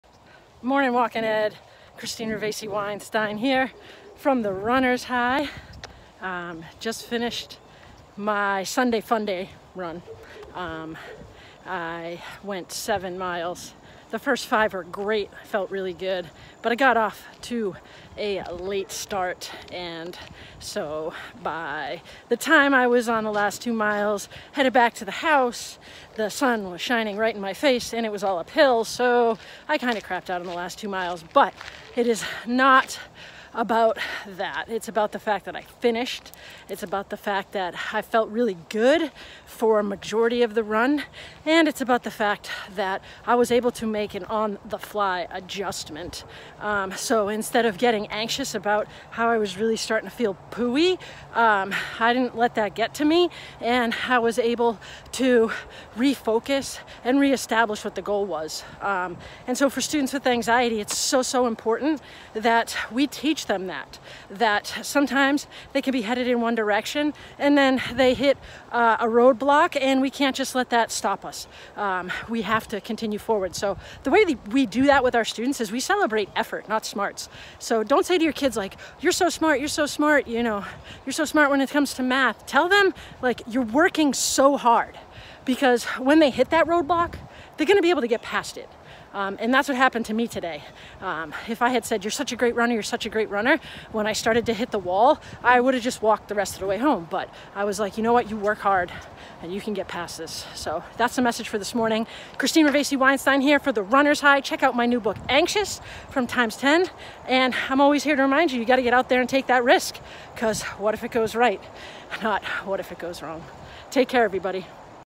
Celebrate Effort, Not Smarts
When we celebrate effort and not smarts, we enable our students to overcome challenges.